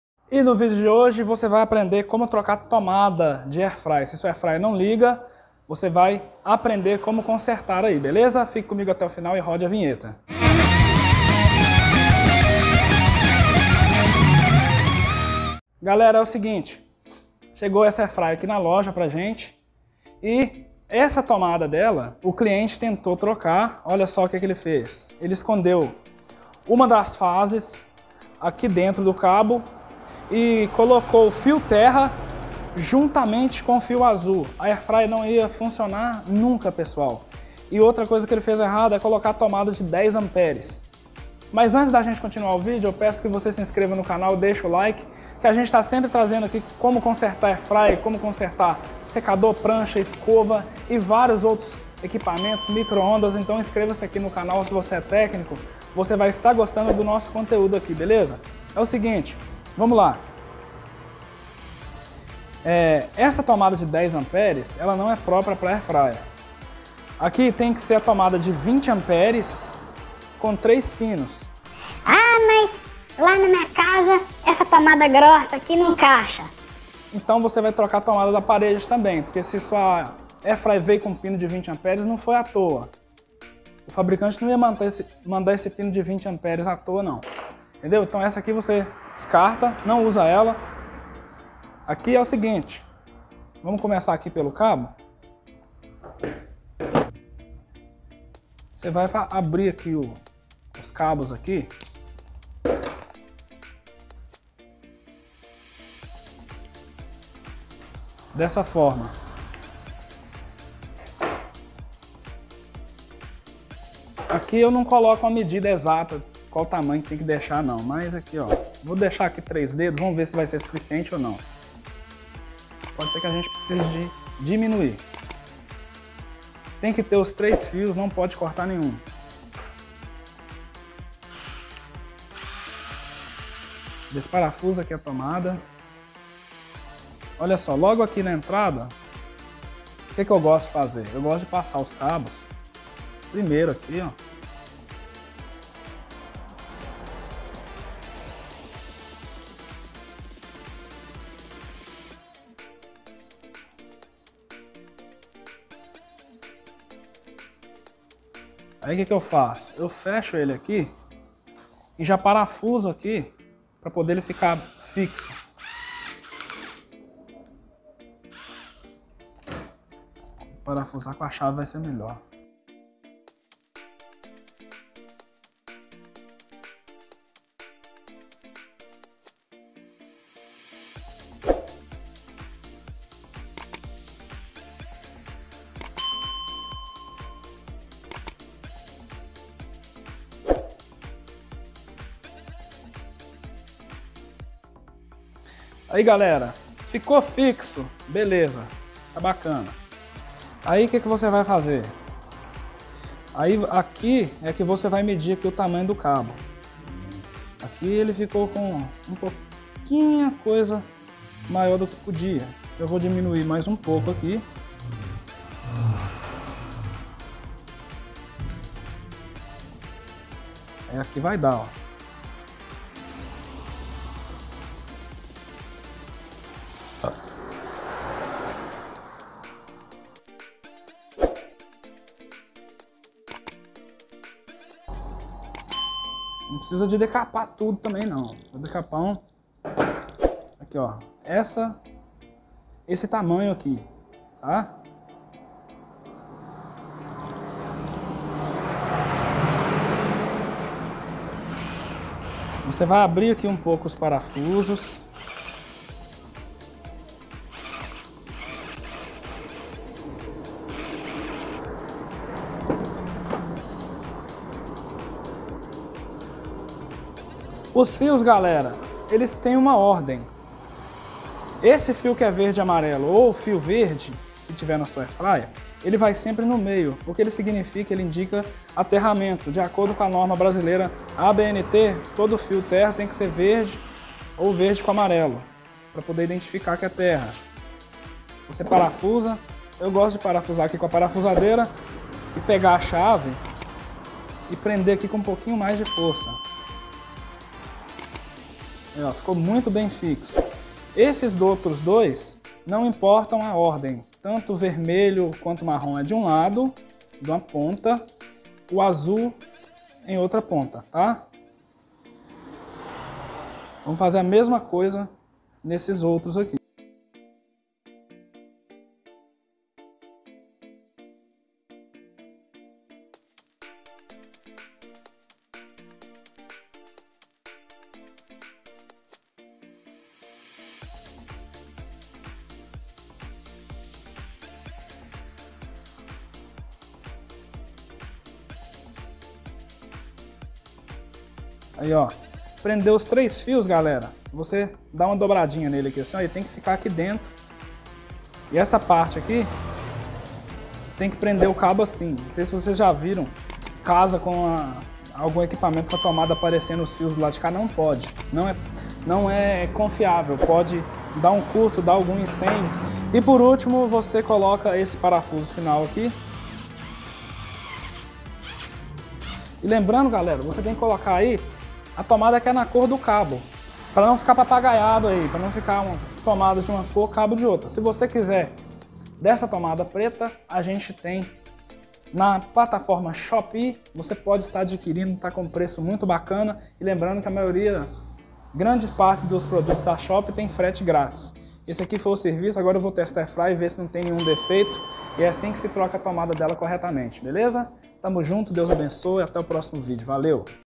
Como Trocar instalar Tomada nova 3 Pinos
Como Trocar Tomada nova 20A 3 Pinos de Friradeira Air Fryer Microondas e outros eletrodomésticos (Plug Macho)
serve para vários equipamentos
microondas, fritadeira churrasqueira elétrica etc
aprenda como instalar ou trocar corretamente